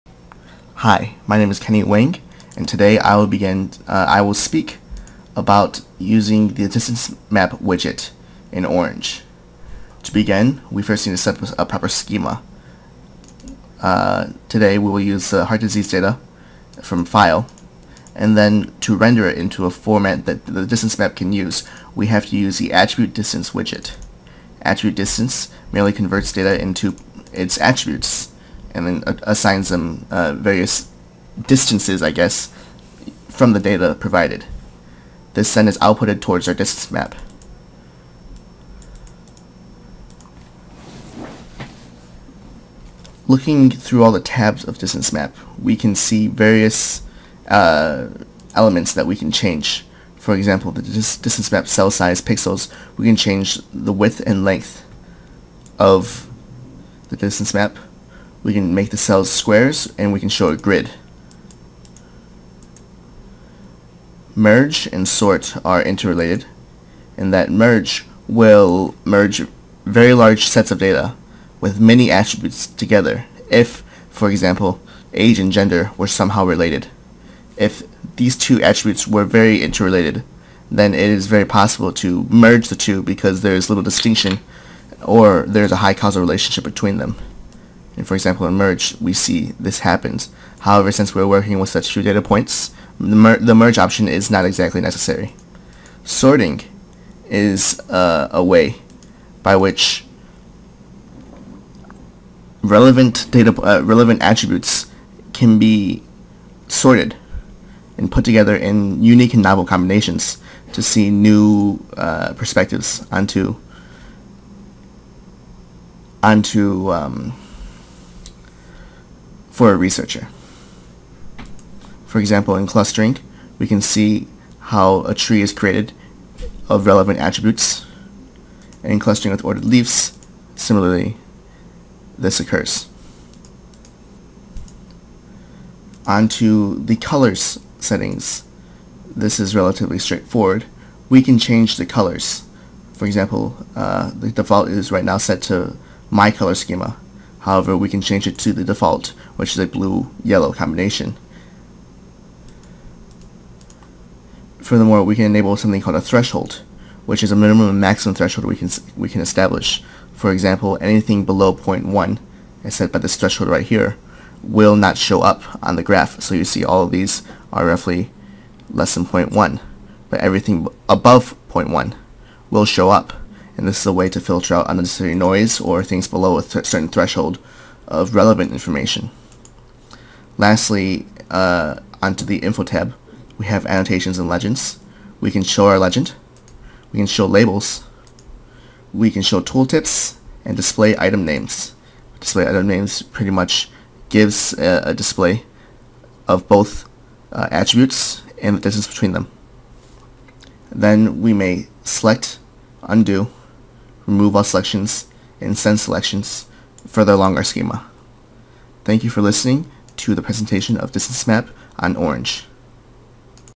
Orange Canvas Widget Demonstration - Distance Maps
A light review of Distance Maps widget in Orange.